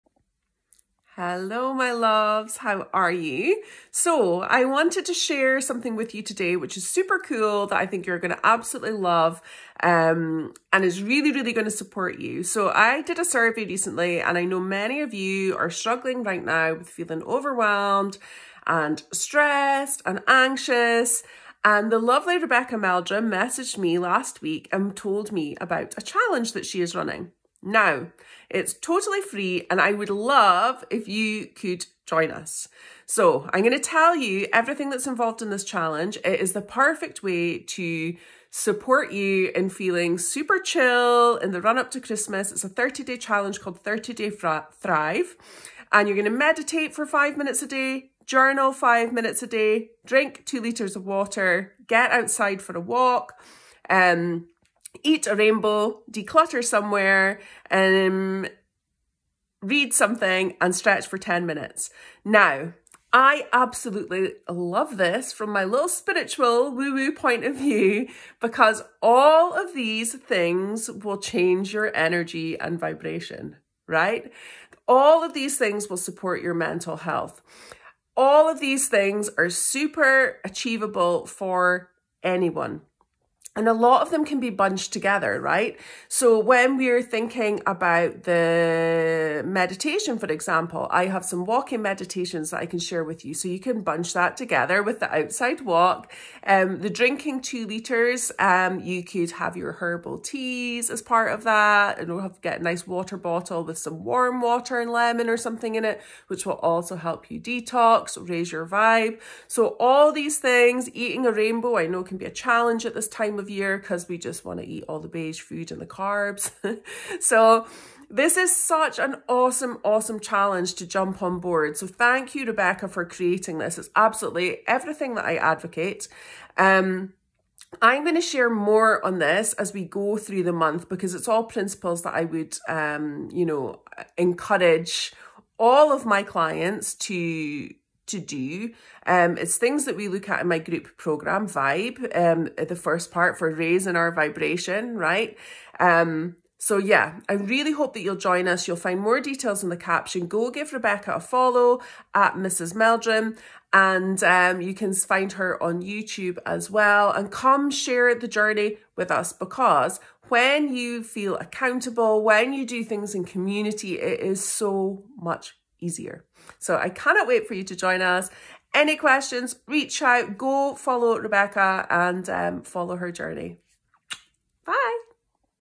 30 Day Thrive Challenge
30 Day Thrive Challenge

Hey loves the lovely Rebecca  @mrsmeldrum has come up with a genius way of supporting us for the next 30 days. I love a challenge and certainly love simple, practical ways to support your mental, emotional, physical and spiritual health. I know life can feel a bit heavy right now and this challenge is such a great way to bolster your mood, energy, vibration and have fun!

The challenge is
Meditate 5mins
Journal 5 mins
Stretch 10 mins
drink 2 litres of water
Get outside for a walk
Eat a rainbow
Declutter somewhere
Read Something

This is free, come follow along on instagram @mrsmeldrum or you can also find Rebecca’s blogs on YouTube

I’ll be sharing hints, tips and trips to support you along the way as I join in too. So much easier in community for accountability. Tag me and Rebecca as you go for a bit of accountability.

Let’s thrive 💕💕💕💕

Love Sus xxx